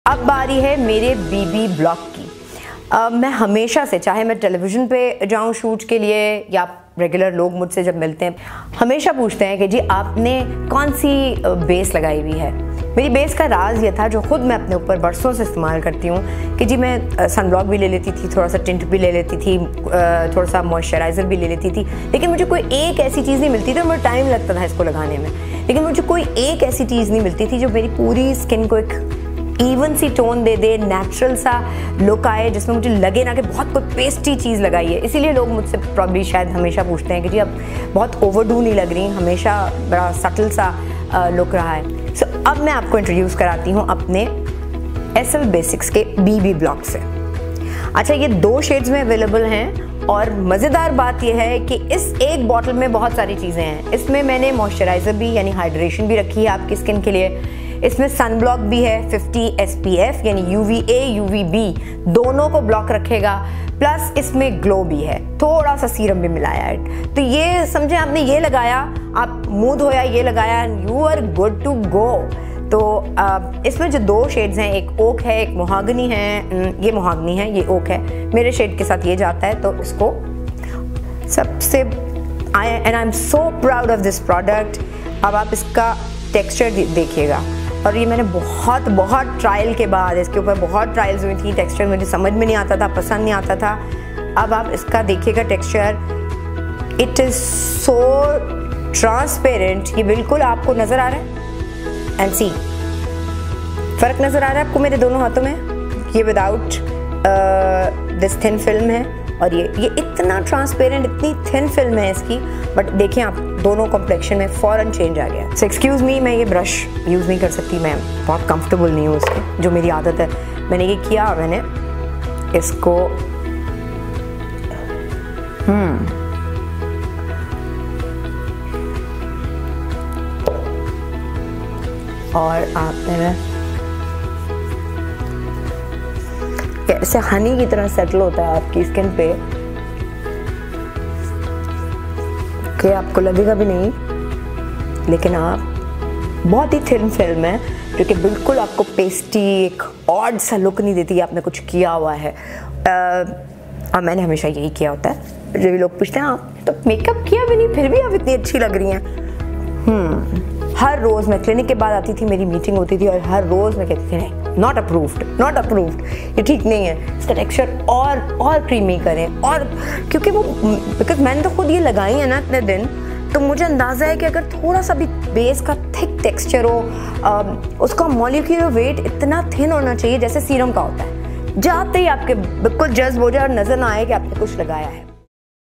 SL Basics presents BB Block by Dr Shaista Lodhi
SL Basics presents new and improved BB Block. So grab yours now and glow all day
BB block is a versatile combination of products such as moisturizer, primer, SPF and is beneficial for your skin for everyday use. It also provides good coverage with added skincare benefits❤

HYDRATION●PROTECTION●COVERAGE